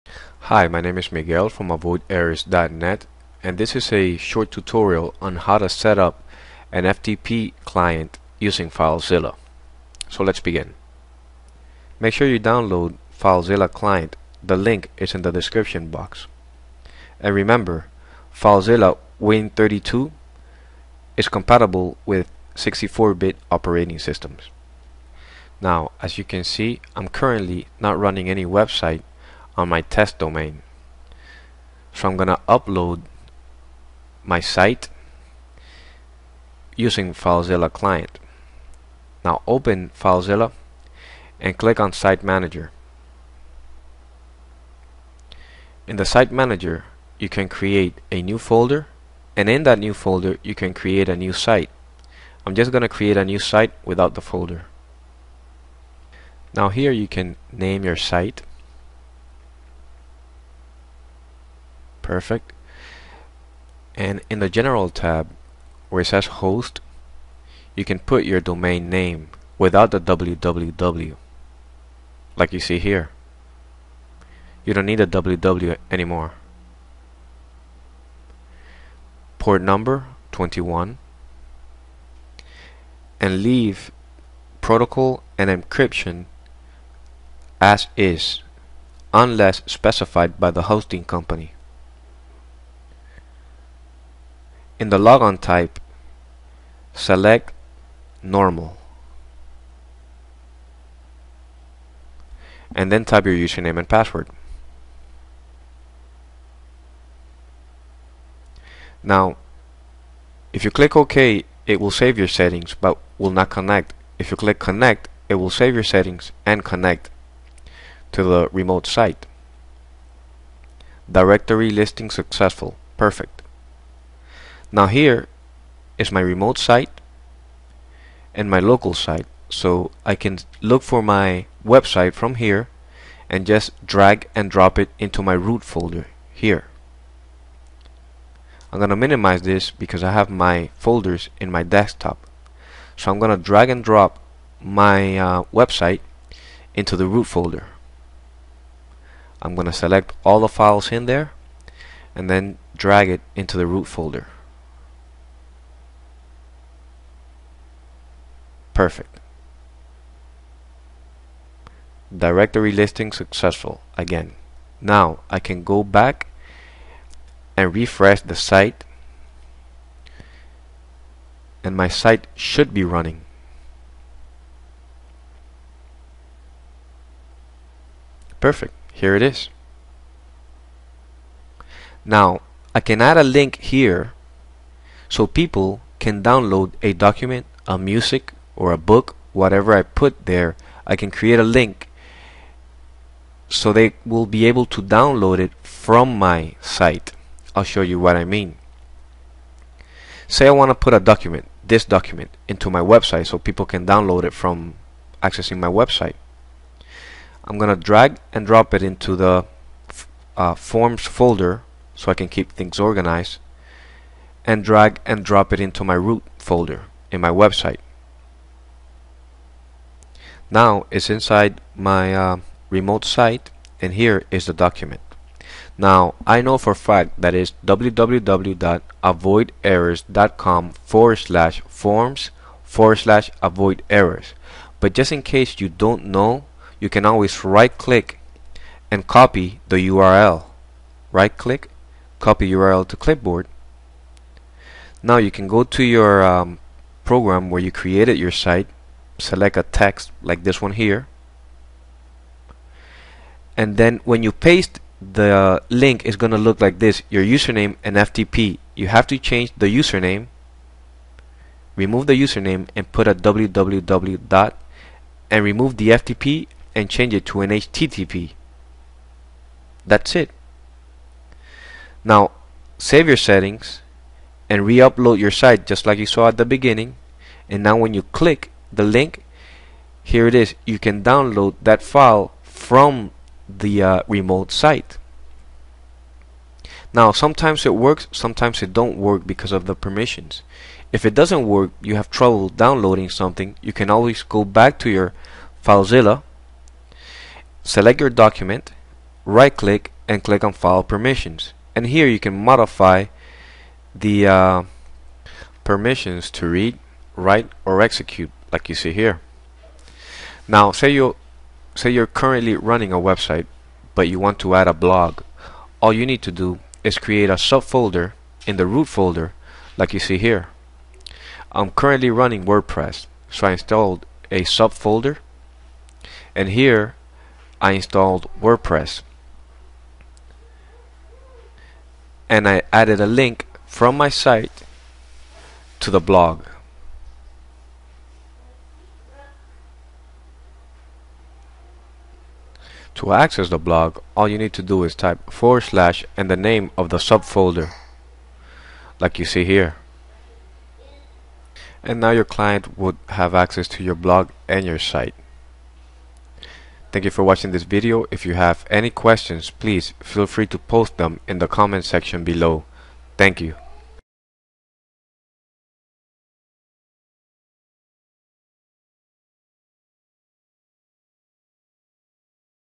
How to Set up and use FileZilla FTP Client
FileZilla Client tutorial. In this tutorial you will learn how to get started with FileZilla as your FTP Client.